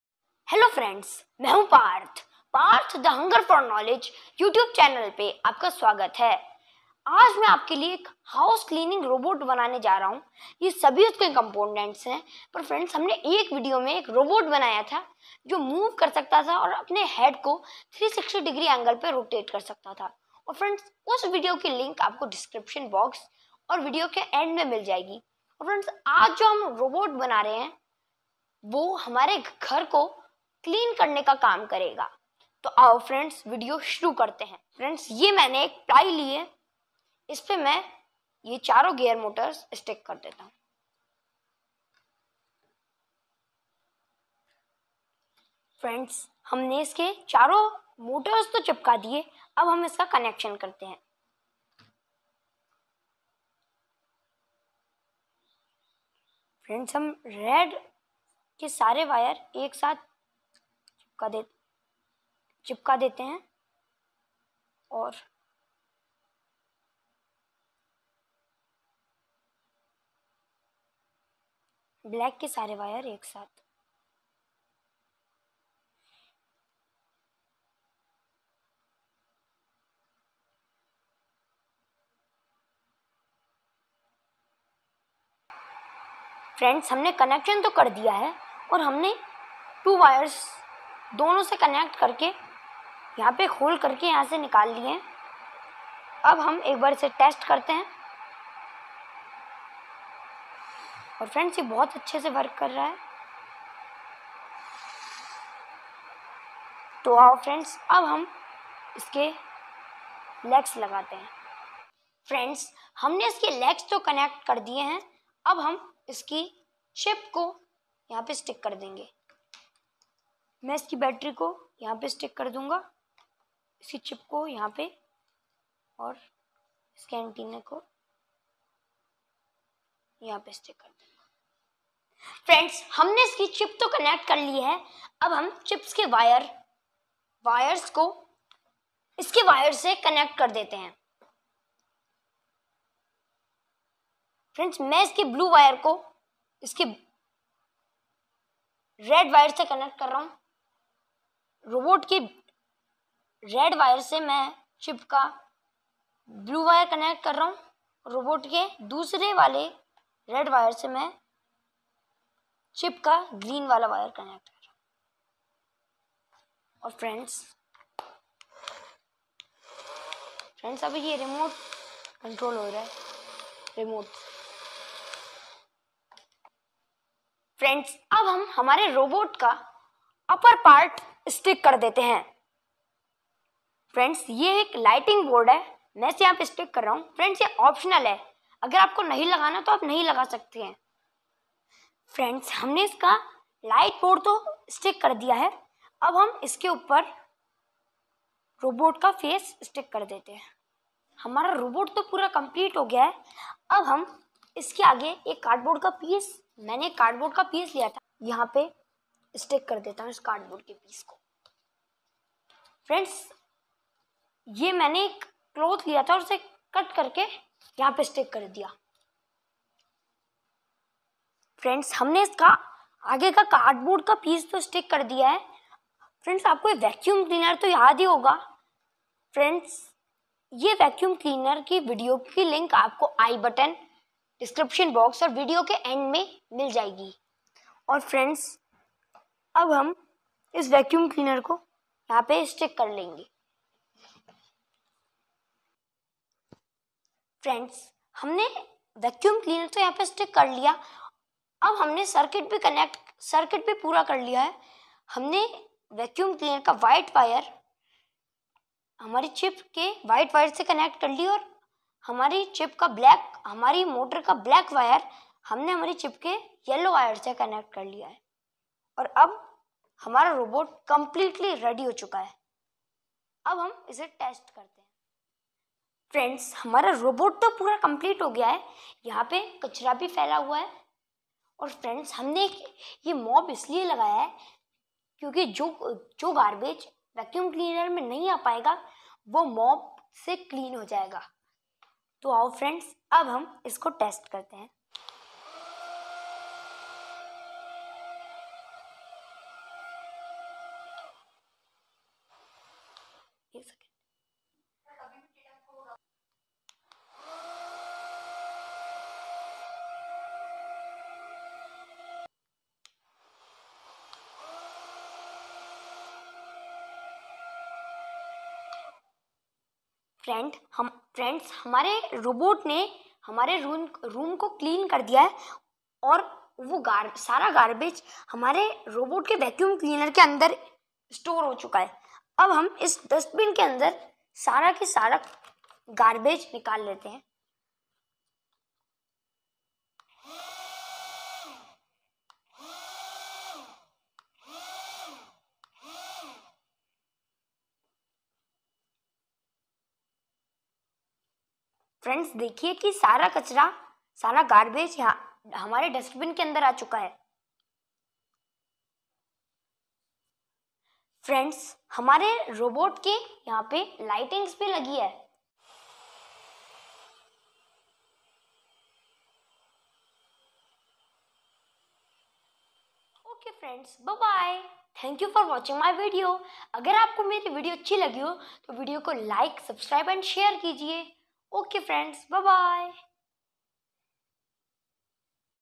How to make a robot with a vacuum cleaner
How to make a robot with a vacuum cleaner. Earlier in the last videos, we have made a Robot and a vacuum cleaner and we understood the principle on which a vacuum cleaner works. in This video, we will make a robot with a vacuum cleaner that can clean our house.